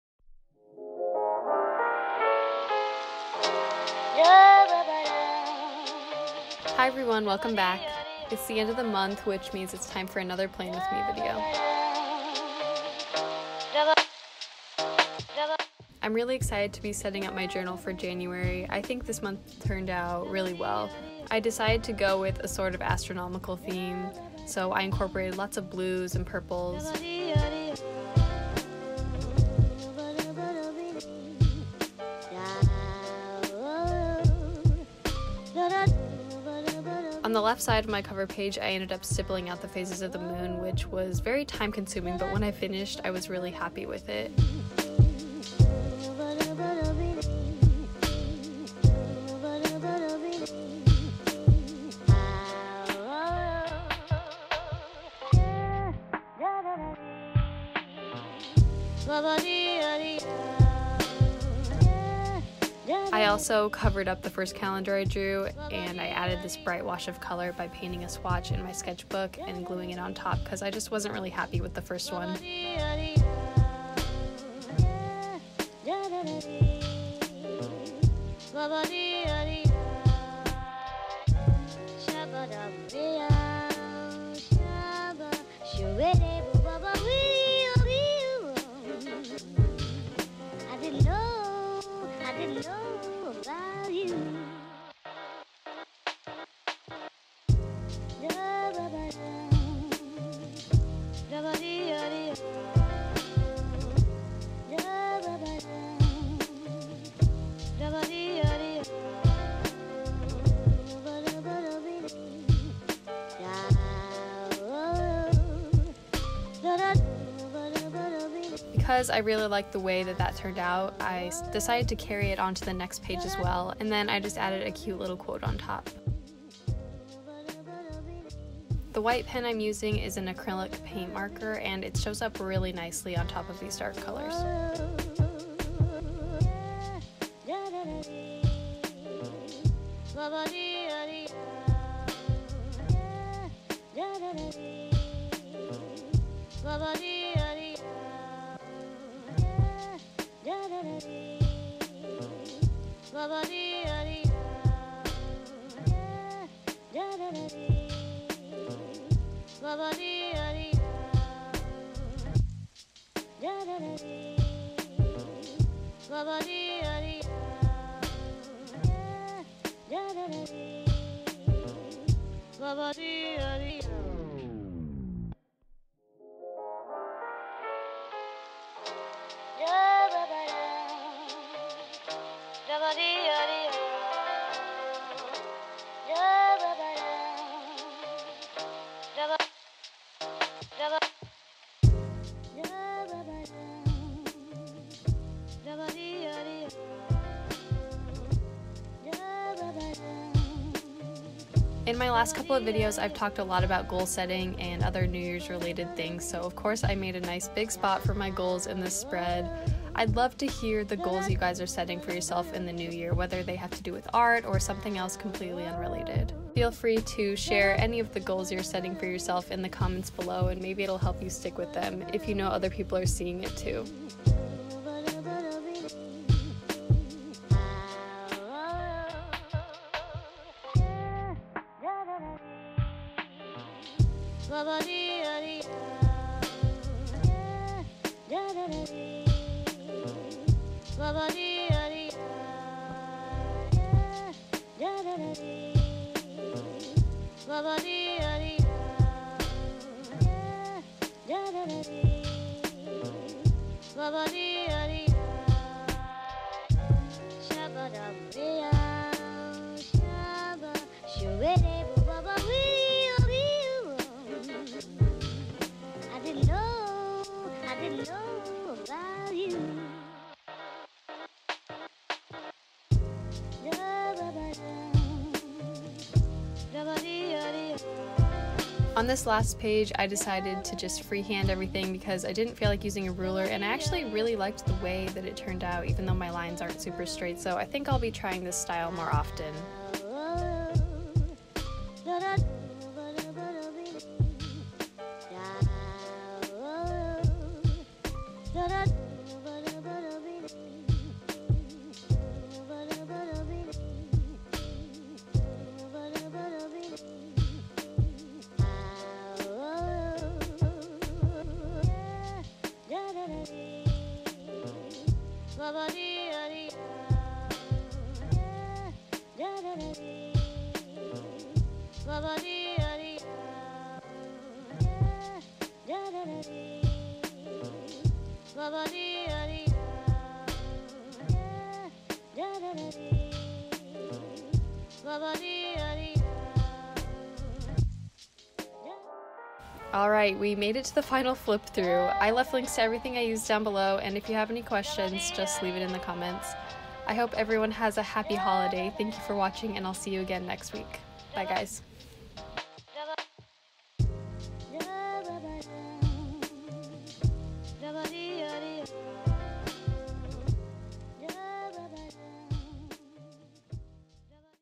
Plan With Me! January Bullet Journal Set-up Ideas Space Theme
My January 2018 bullet journal set up.

Supplies used:
Leuchtturm1917 Dotted Journal
Pentel Waterbrushes
Tombow Fudenosuke pen
Uni-Ball UM 153 Signo White Gel Pen
Micron PN
Pigma Graphic 1
Gouache Paint